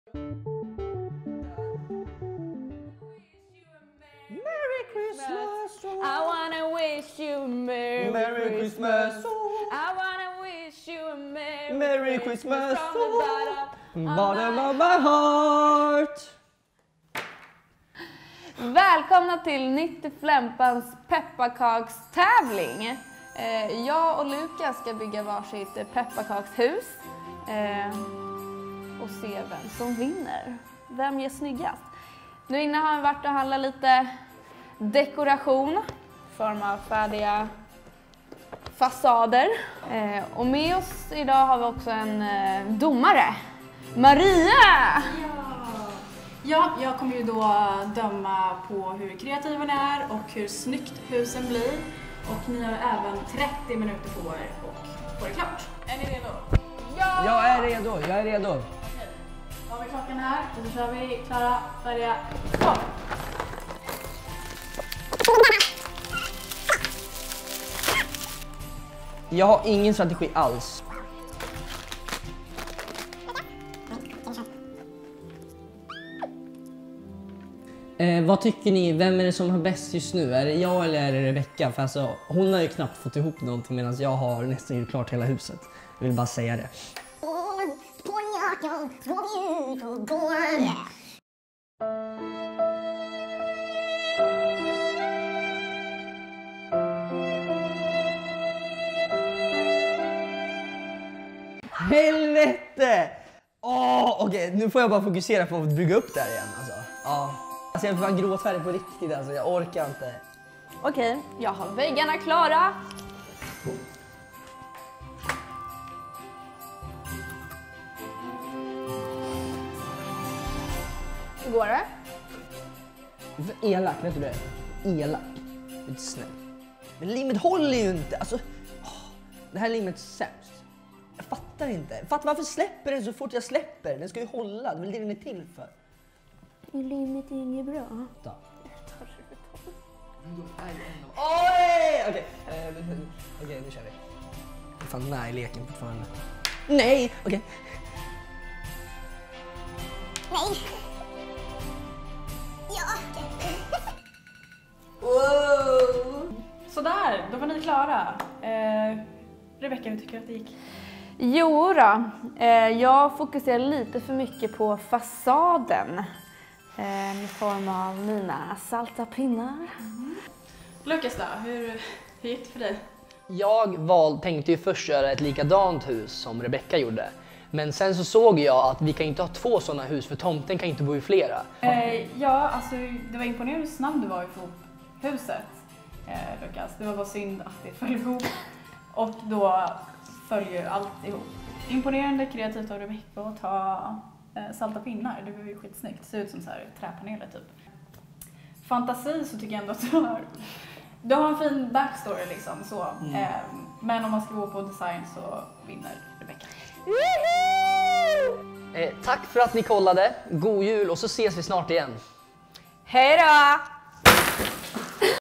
Nytt i Flempan tävlar: Pepparkakshus
Lukas Nordén och Rebecka Rakell från Nytt i Flempan tävlar i pepparkaksbygge. Vem vinner?

Dela med dig av dina tankar i kommentarsfältet nedan!
Följ oss på och sociala medier på @nyttiflempan för fler nyheter!